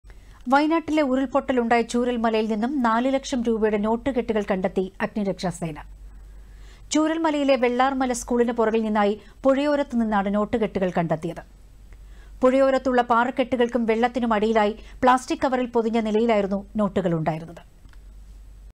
നാലു ലക്ഷം രൂപയുടെ നോട്ടുകെട്ടുകള്‍ ഉരുള്‍പൊട്ടലുണ്ടായ ചൂരൽമലയില്‍ നിന്ന് കണ്ടെത്തി
വയനാട്ടിലെ ഉരുള്‍പൊട്ടലുണ്ടായ ചൂരൽമലയില്‍ നിന്ന് നാലു ലക്ഷം രൂപയുടെ നോട്ടുകെട്ടുകള്‍ കണ്ടെത്തി അഗ്നി രക്ഷാസേന.

ചൂരല്‍ മലയിലെ വെള്ളാര്‍മല സ്കൂളിന് പുറകിൽ നിന്നായി പുഴയോരത്തുനിന്നാണ് നോട്ടുകെട്ടുകള്‍ കണ്ടെത്തിയത്.

പുഴയോരത്തുള്ള പാറക്കെട്ടുകള്‍ക്കും വെള്ളത്തിനുo അടിയിലായി പ്ലാസ്റ്റിക് കവറില്‍ പൊതിഞ്ഞ നിലയിലായിരുന്നു നോട്ടുകളുണ്ടായിരുന്നത്.